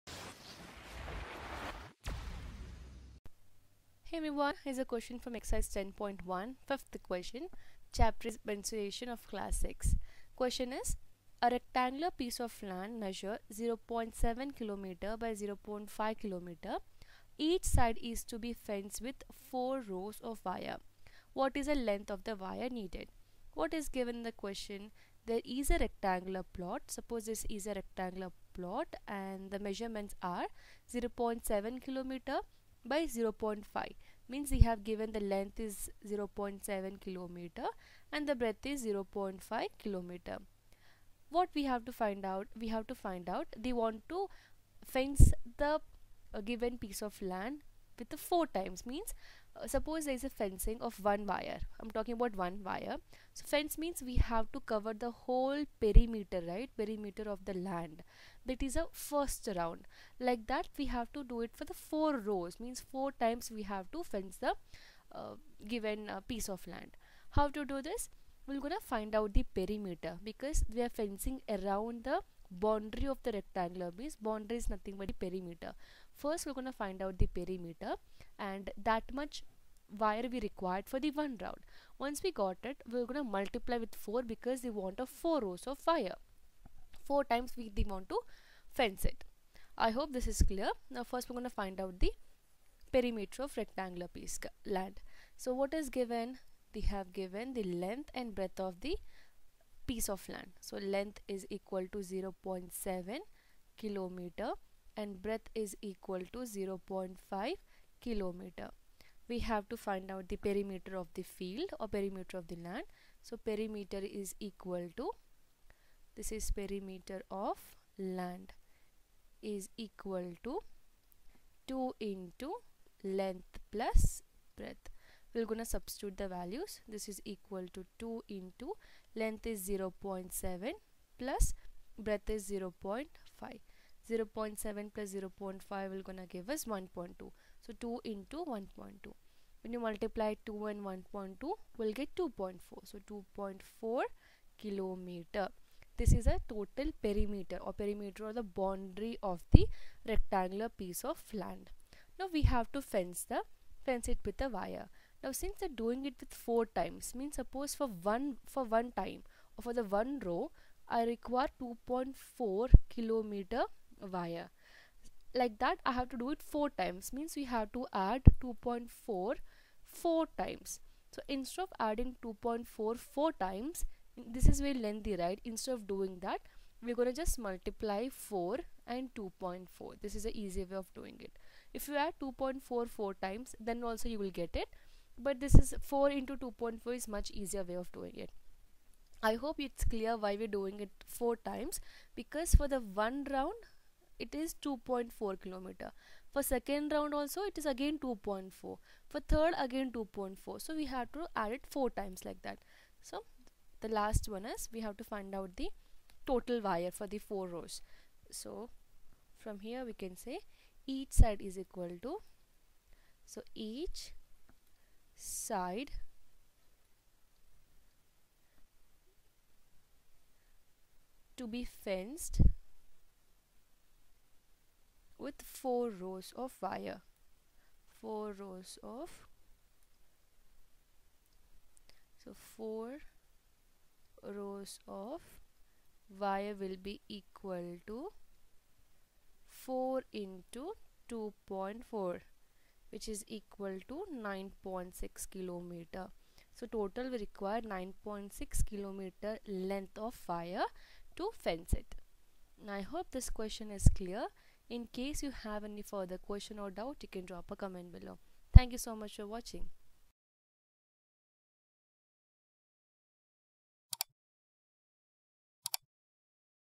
5q Ex10.1 Mensuration Class 6.A rectangular piece of land measures 0.7 km by 0.5 km. Each side......
A rectangular piece of land measures 0.7 km by 0.5 km. Each side is to be fenced with 4 rows of wires. What is the length of the wire needed?